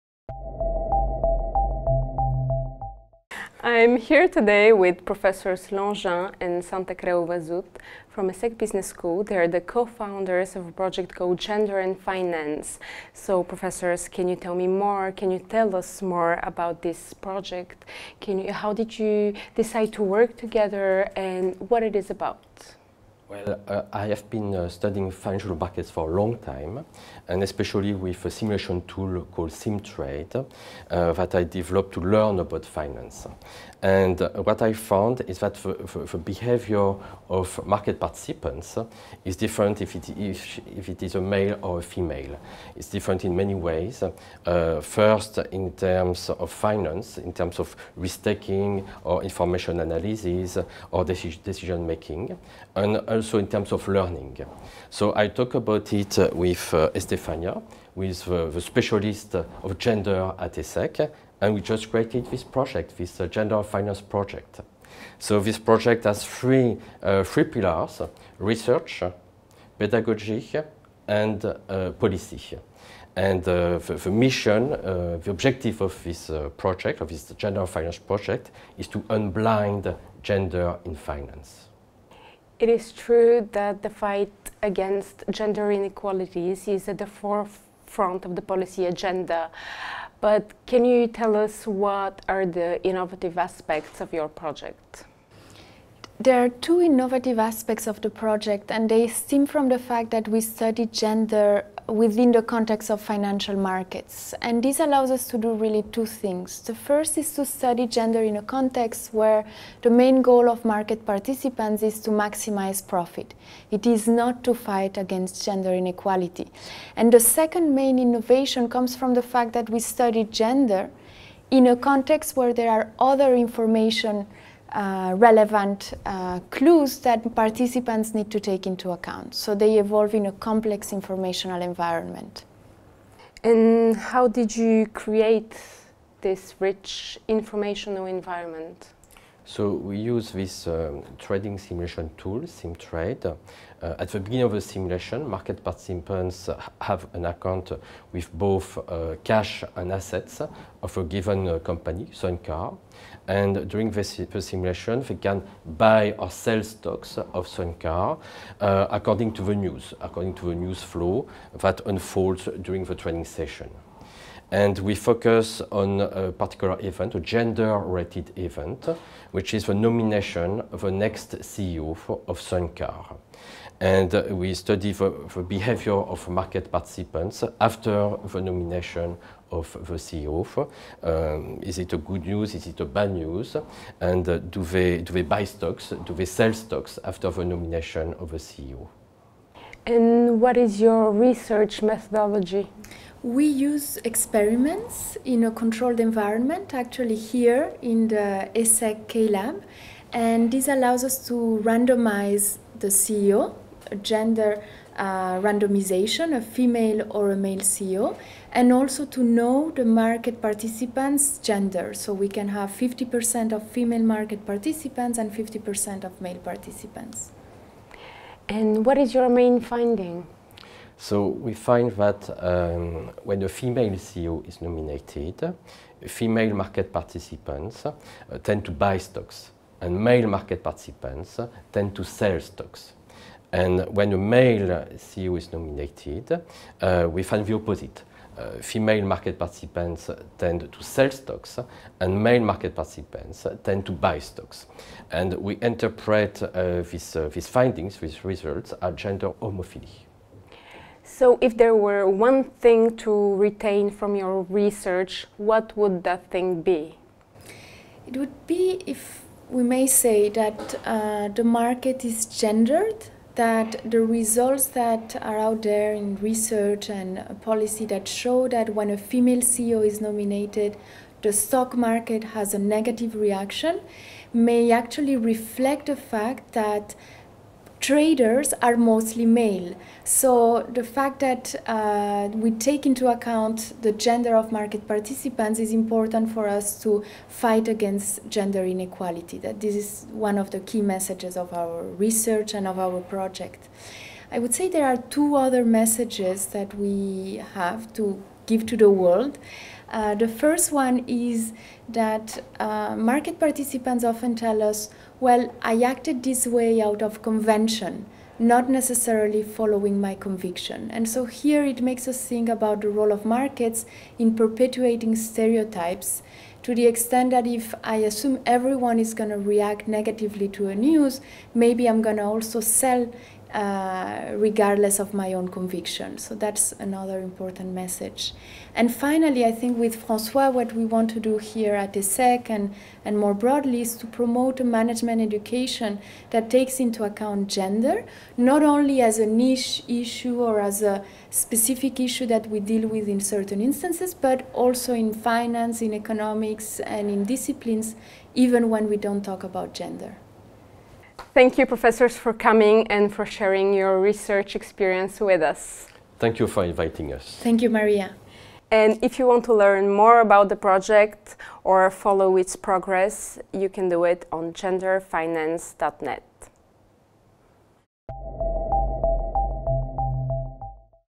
Gender & Finance - Prof. Santacreu-Vasut et Longin
François Longin, Professor of Finance, and Estefania Santacreu-Vasut, Associate Professor of Economics, present the Gender & Finance project and their recent research on the market reaction to the nomination of CEO .